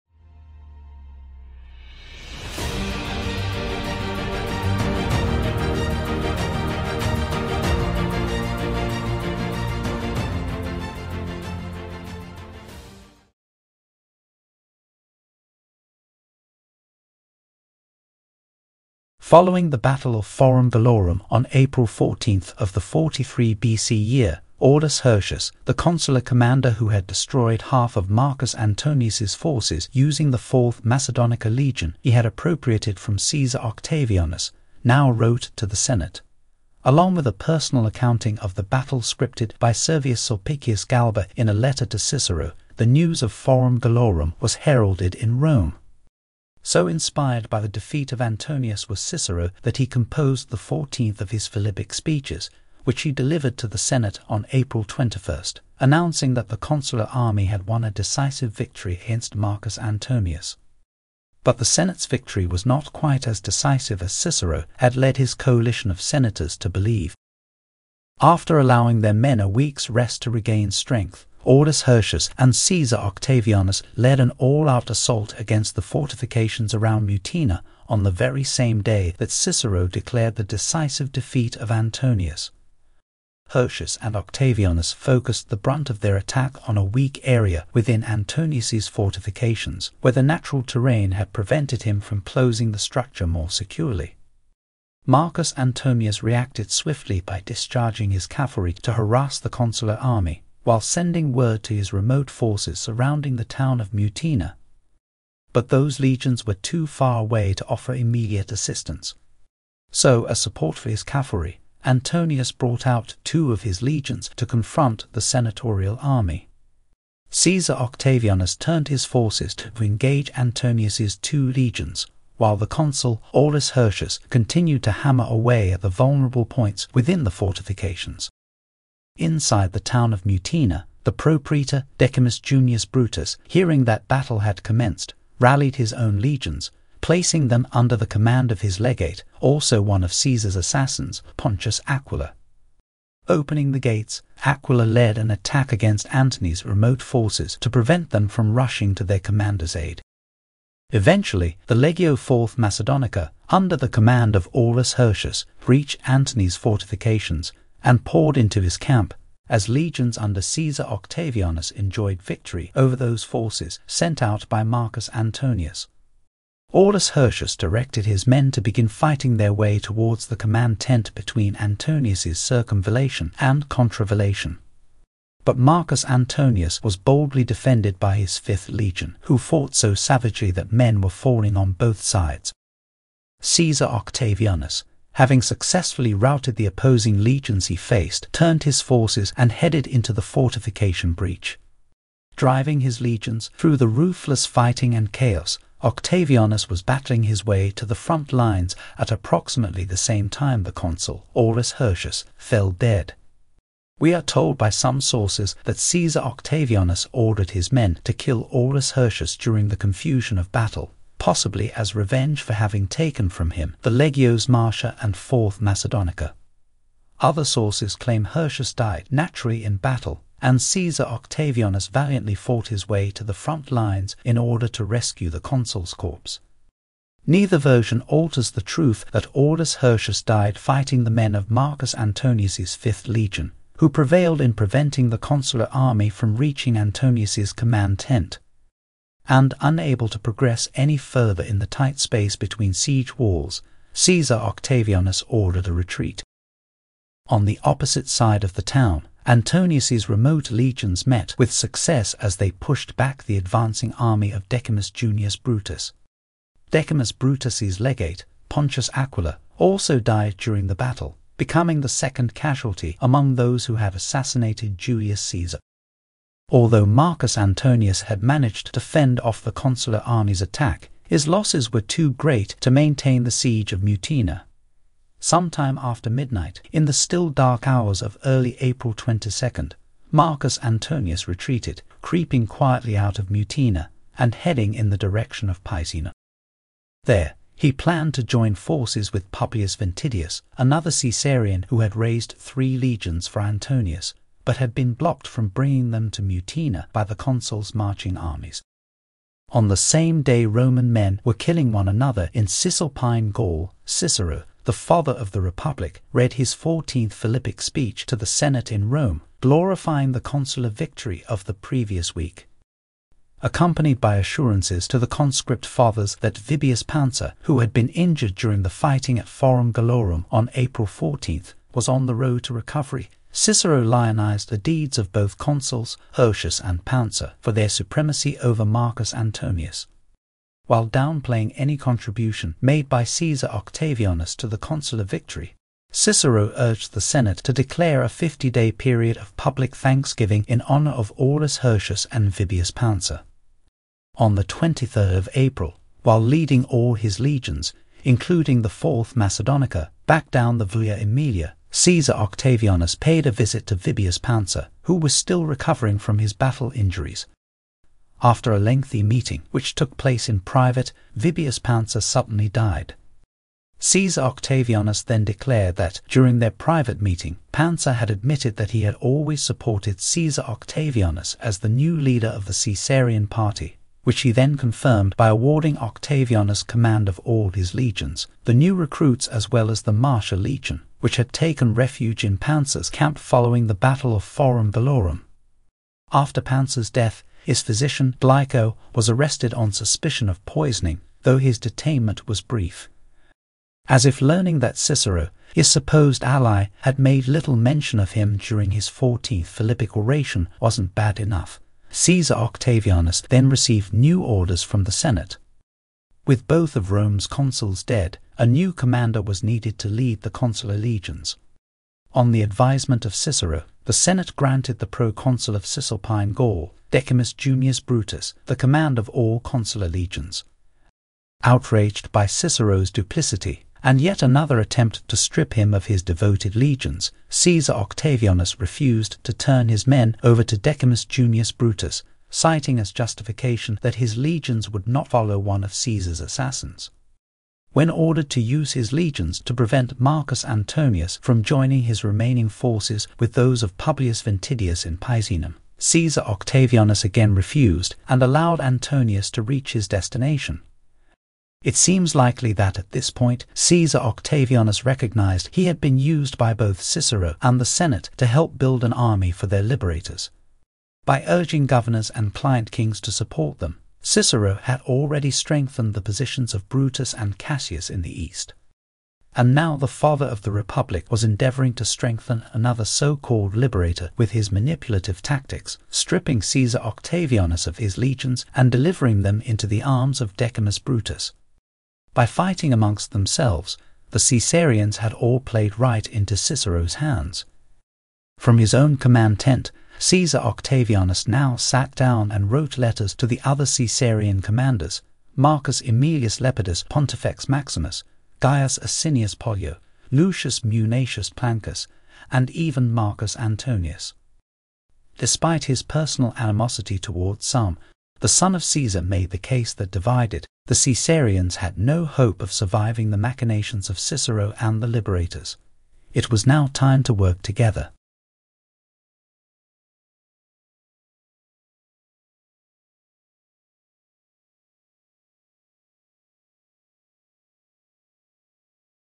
43 BC | Battle of Mutina
After having his forces annihilated at the Battle of Forum Gallorum, Mark Antony takes a beating as the Consular army moves to break the siege of Mutina.  But Antony finds reprieve from an unlikely source!